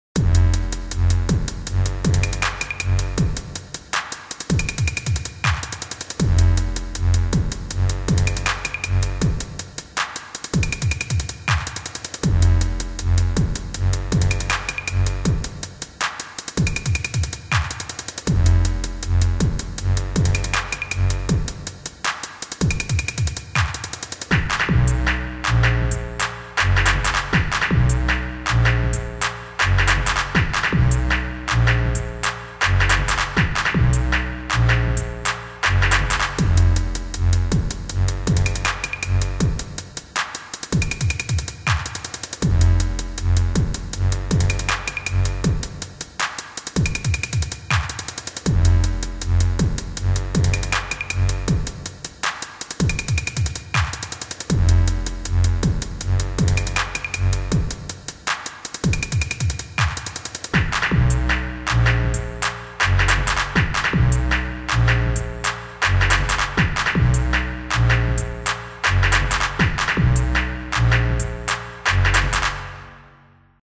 grim instrumental
booted up my old dell from highschool, found this grime instrumental i made in 2005. ha.